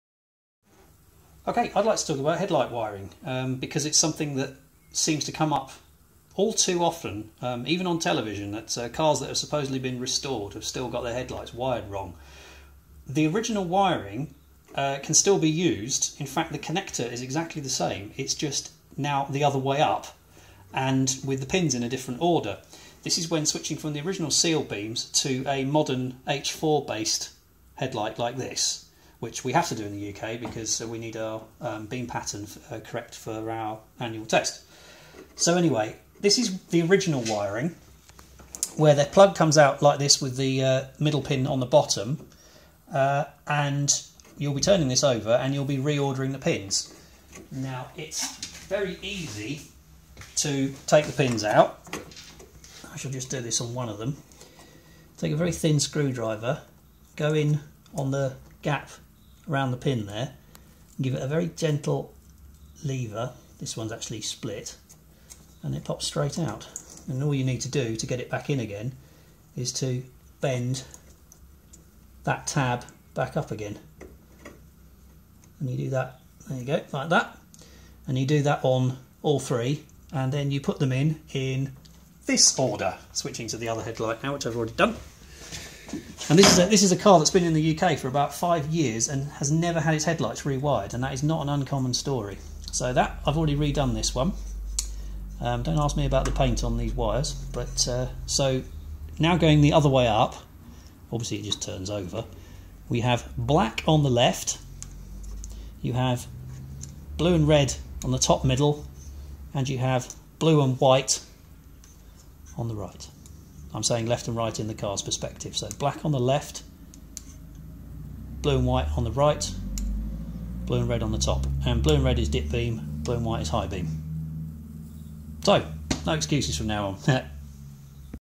DeLorean H4 headlight wiring how 2
How to re-wire Delorean headlights for H4 bulbs.